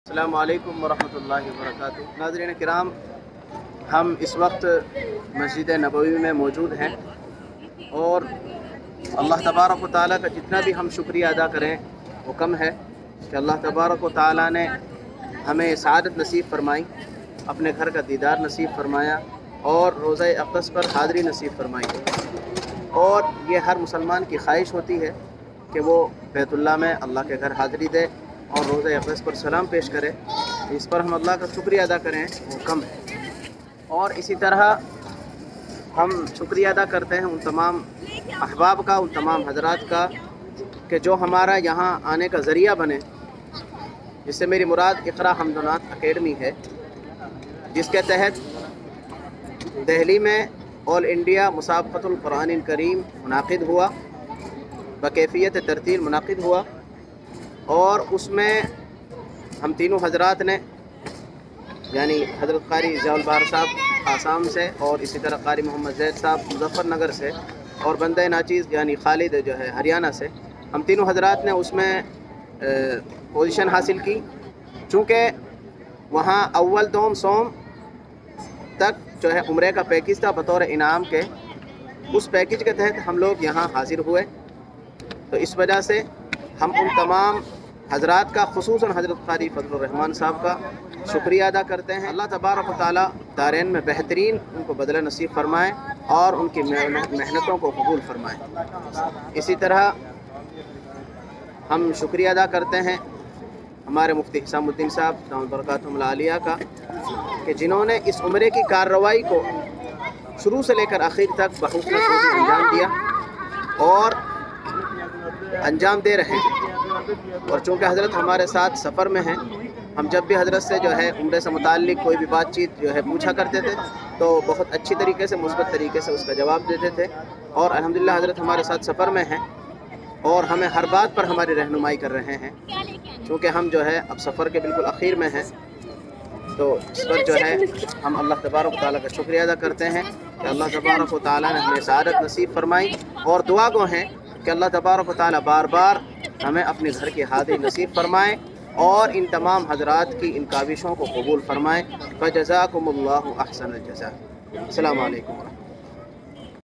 Thanks to Iqra Hamd Naat Academy Delhi || Madina Munwwarah me
Alhamdulillah❤️ Madina munwwarah me || Thanks Iqra Hamd Naat Academy Delhi | Qari Jiaul Bahar.

Masjide Nabawi me Tilawaat karte huye || Qari Jiaul Bahar.

Qari Jiaul Bahar Facebook.